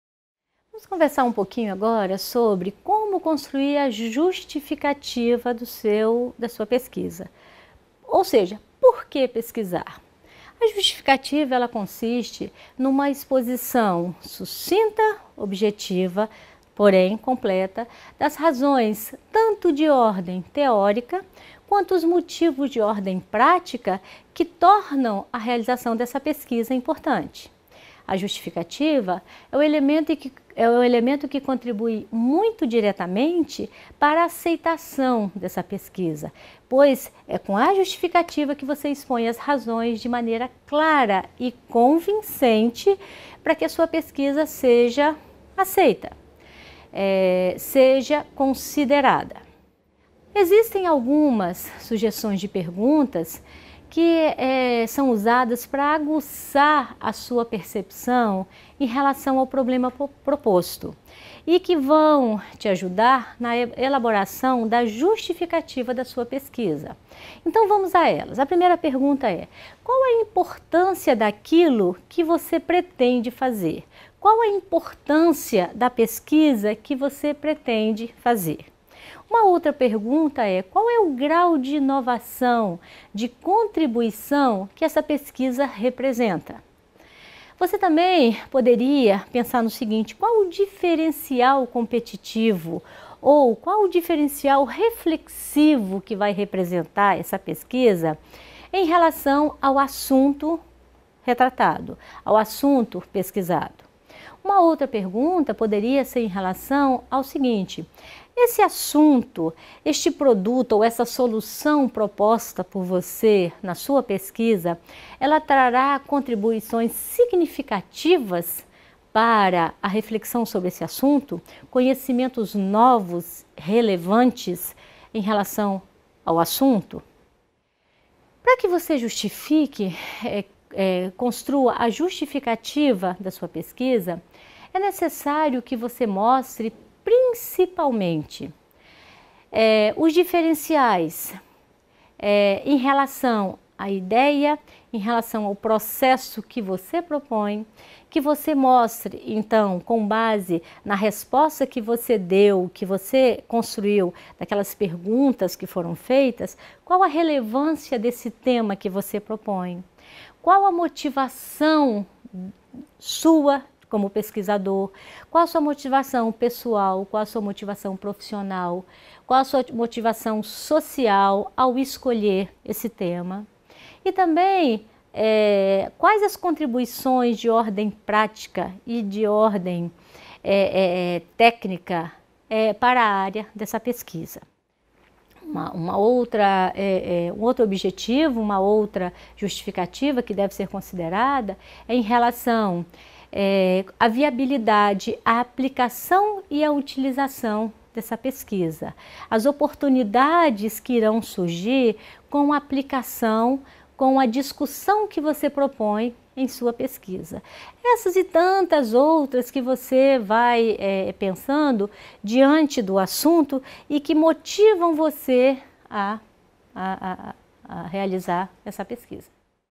Metodologia de pesquisa - Modulo 3 - Justificativas [Libras]
Curso: Pós-Graduação em Práticas Pedagógicas
Disciplina: Metodologia de Pesquisa em Educação
Professor: Esther Ortlieb
Semana: 3